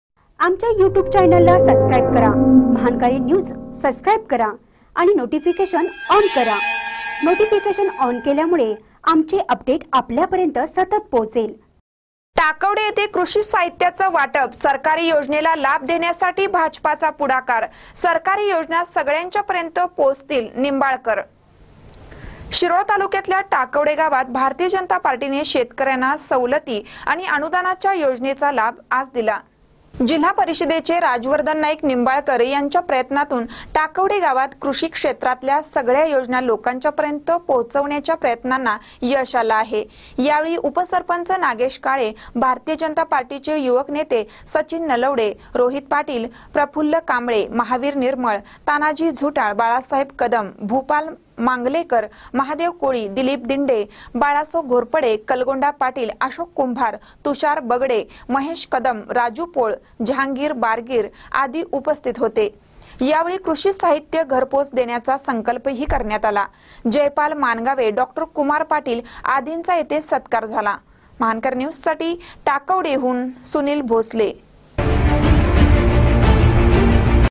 कृषी साहित्याचं वाटप सरकारी योजनेला लाभ देण्यासाठी भाजपाचा पुढाकार
टाकवडे येथे कृषी साहित्याचं वाटप सरकारी योजनेला लाभ देण्यासाठी भाजपाचा पुढाकार सरकारी योजना सगळयांच्यापर्यत पोहोचतील निंबाळकर